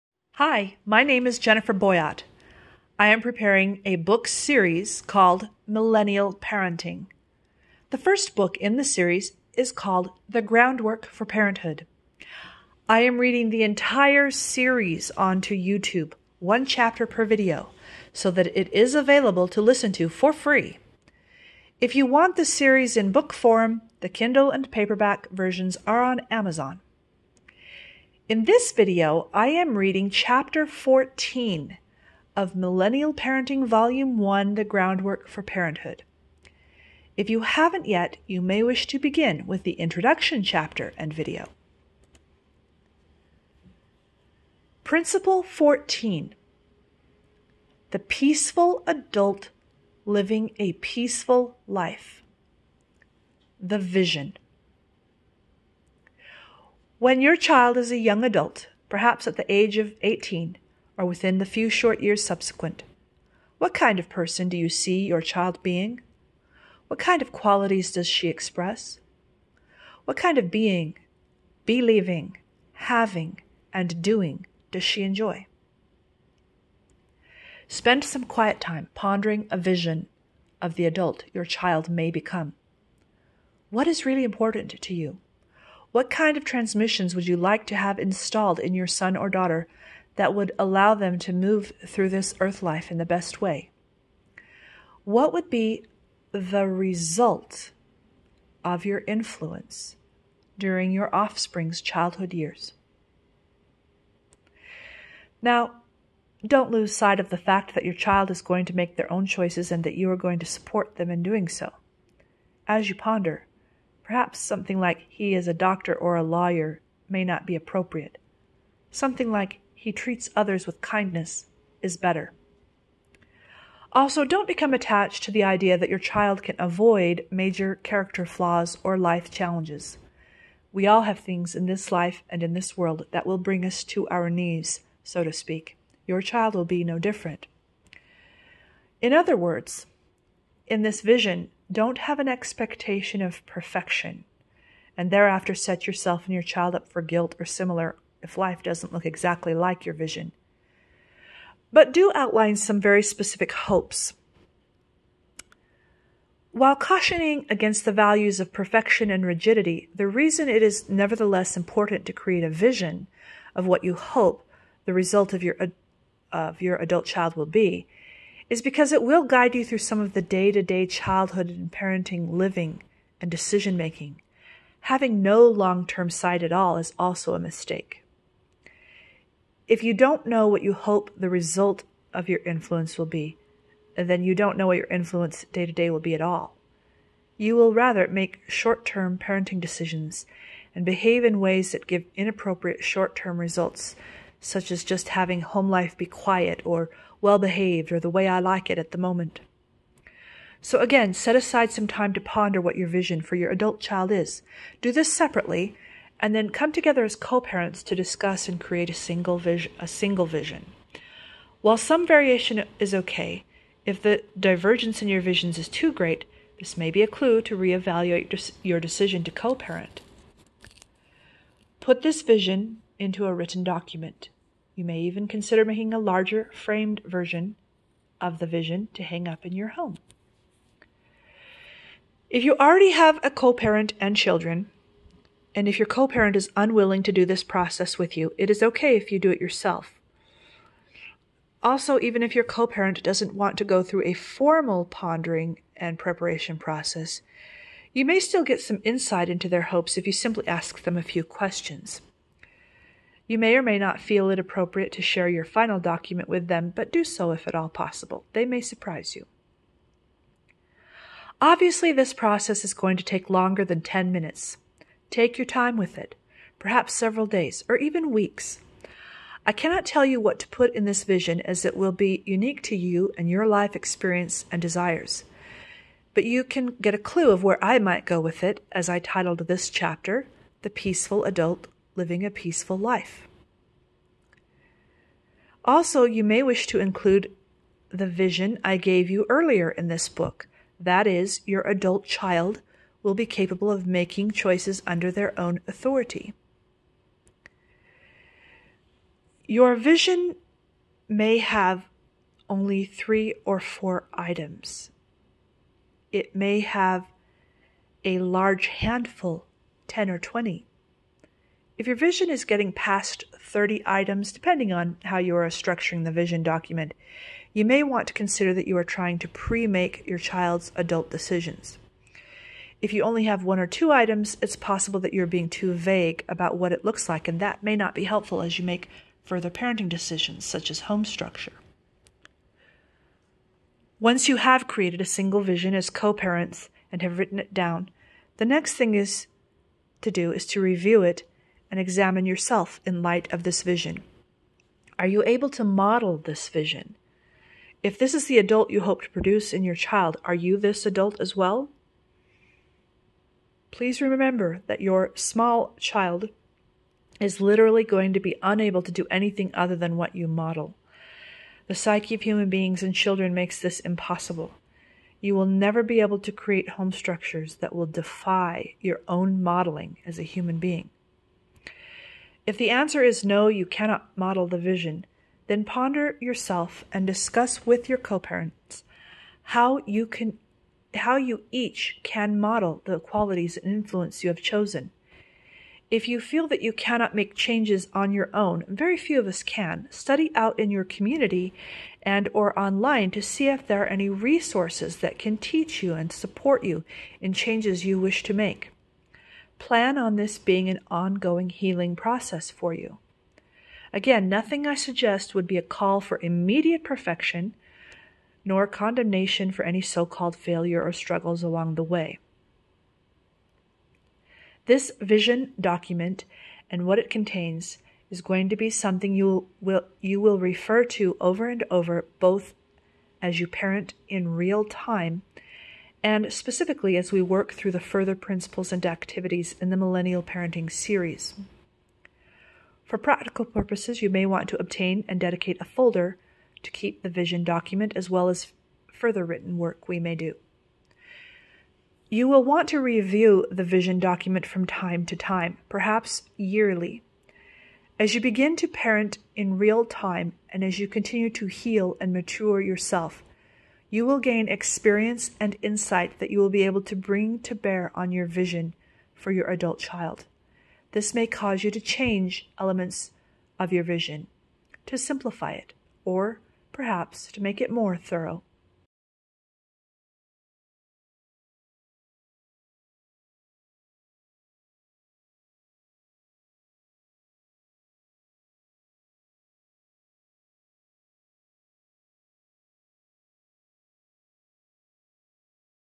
Create your parenting Vision (Chapter 14) (Milliennial Parenting Volume 1)
Hi. My name is Jennifer Boyatt. I am preparing a book series called Millennial Parenting. The first book in the series is called The Groundwork for Parenthood. I am reading the entire series on to YouTube, one chapter per video, so that it is available to listen to for free. If you want the series in book form, the Kindle and paperback versions are on Amazon. In this video I am reading Chapter 14 of Millennial Parenting Volume 1: The Groundwork for Parenthood. If you haven't yet, you may wish to begin with the Introduction chapter and video.

[Millennial Parenting Volume 1]